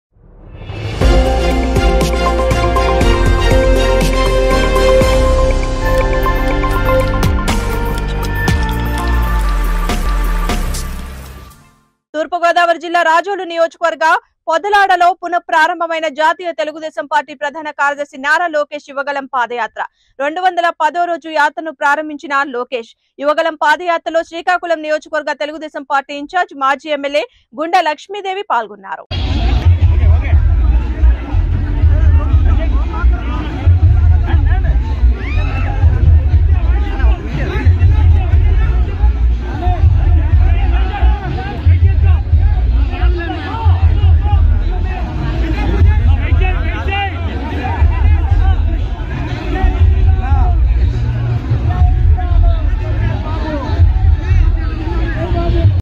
రాజోలు నియోజవర్గం పొదలాడలో పునఃప్రారంభమైన జాతీయ తెలుగుదేశం పార్టీ ప్రధాన కార్యదర్శిmitv
రాజోలు నియోజవర్గం పొదలాడలో పునఃప్రారంభమైన జాతీయ తెలుగుదేశం పార్టీ ప్రధాన కార్యదర్శి